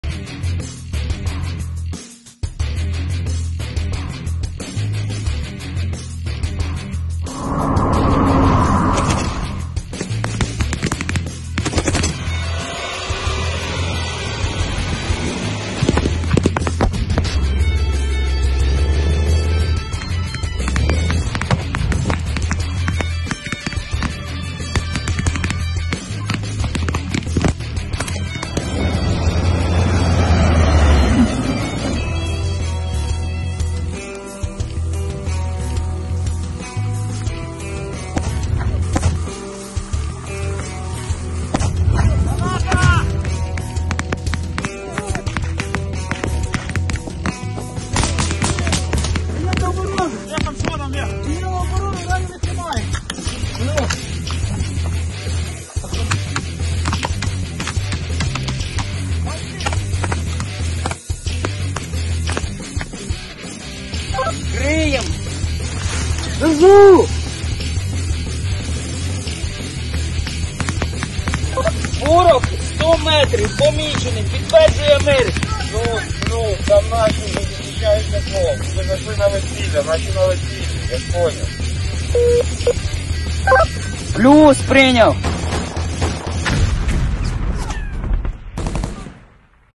Ukraine war
Russian military intervention in Ukraine began in February 2014 after the victory of Euromaidan and the change of power in the country, when Russian troops occupied the Crimean peninsula - the regions of Ukraine Autonomous Republic of Crimea and Sevastopol - and Russia annexed them.

Exactly one year ago, a full-scale Russian invasion of Ukraine began. Ukrainian cities woke up from rocket attacks, the enemy broke through the border in several directions and tried to land near Kiev to quickly capture the capital.